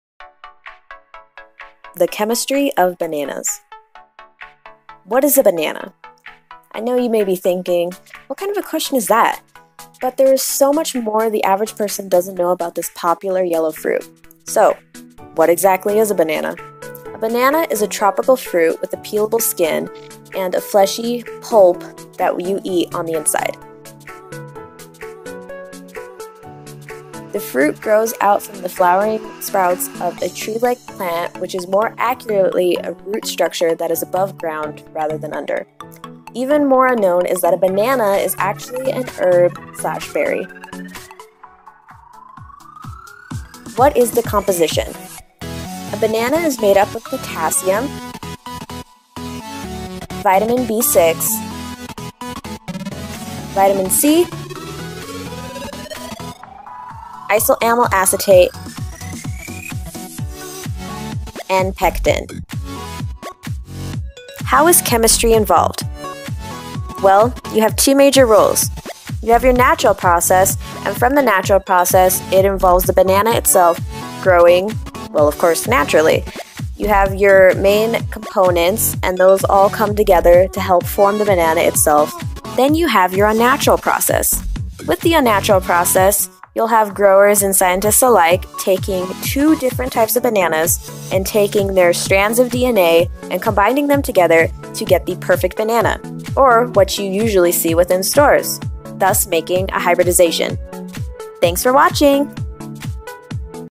The Chemistry Of Bananas - Caitlin J.
Caitlin explains the chemistry of Bananas in this high school chemistry project.